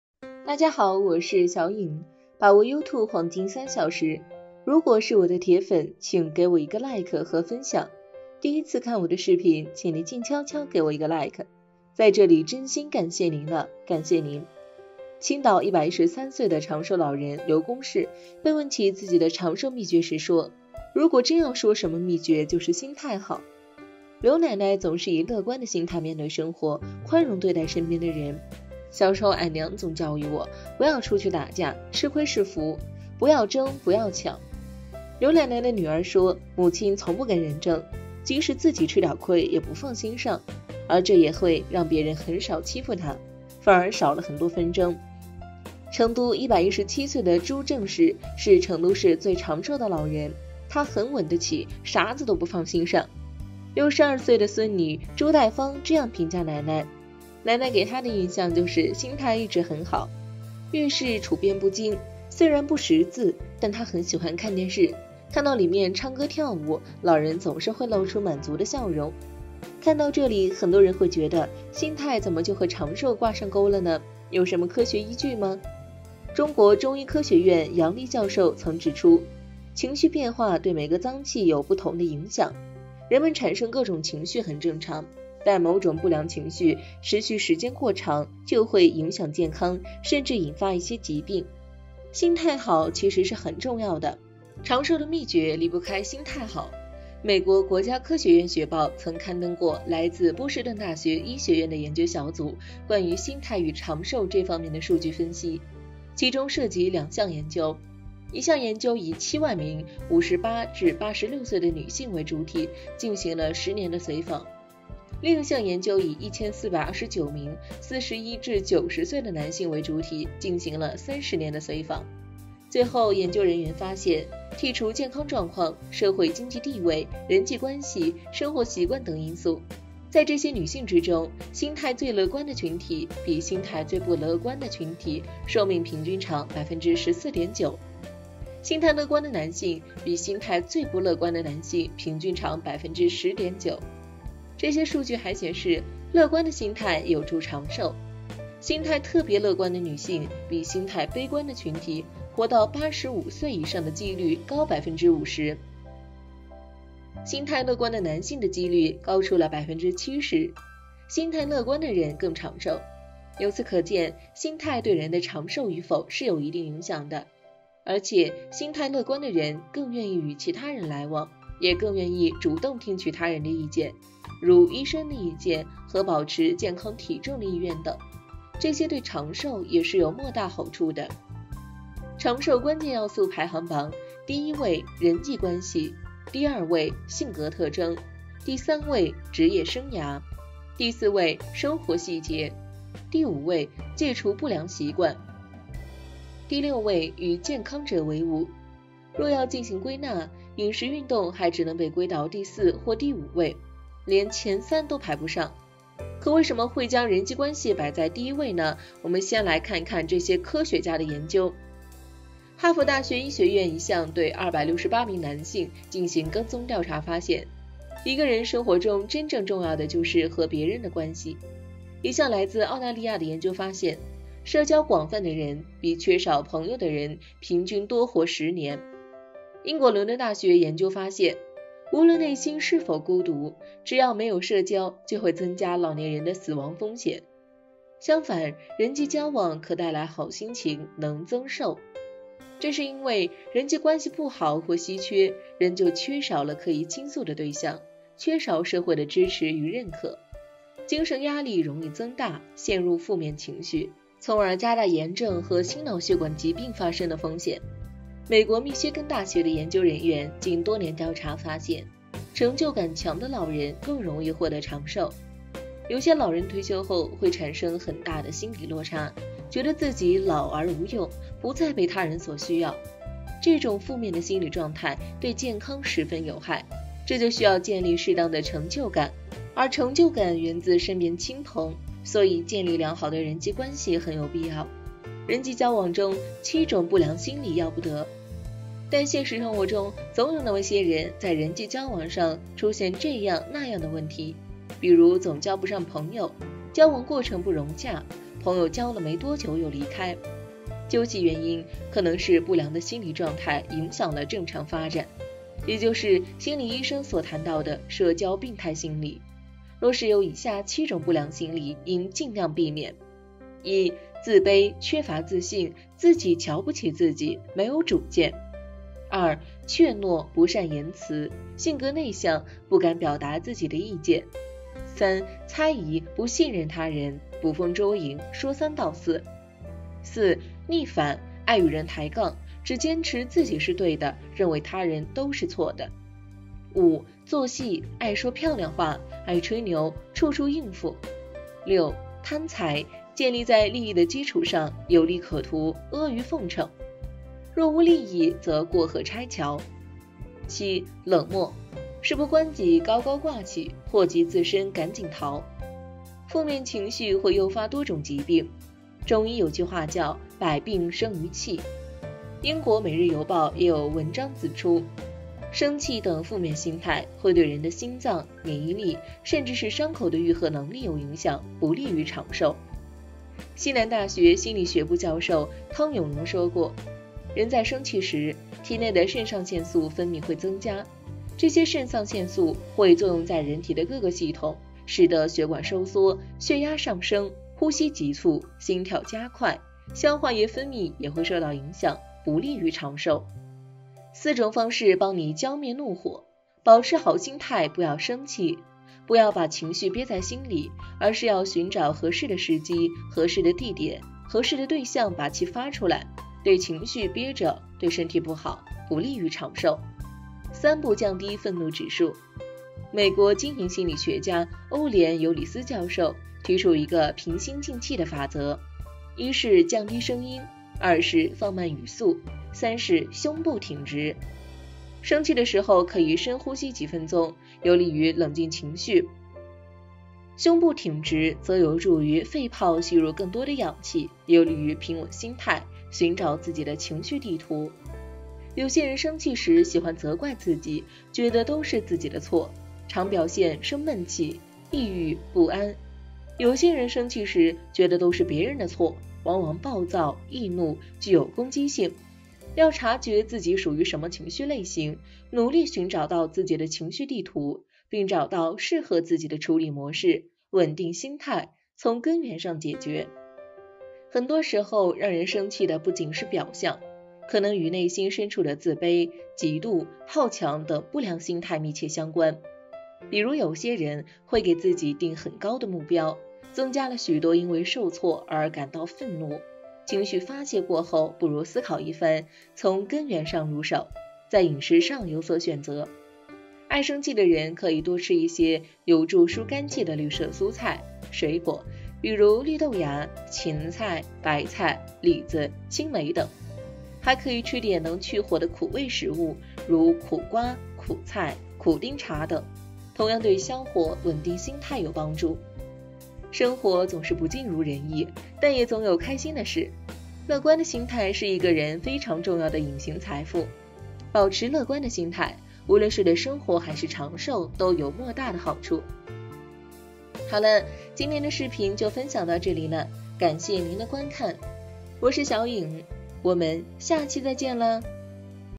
長壽秘訣已破解，竟無關飲食煙酒和運動！醫生直言：主要還得看它，無奈沒多少人知道【小穎養生】
多謝你支持 有空給我一個Like 🙏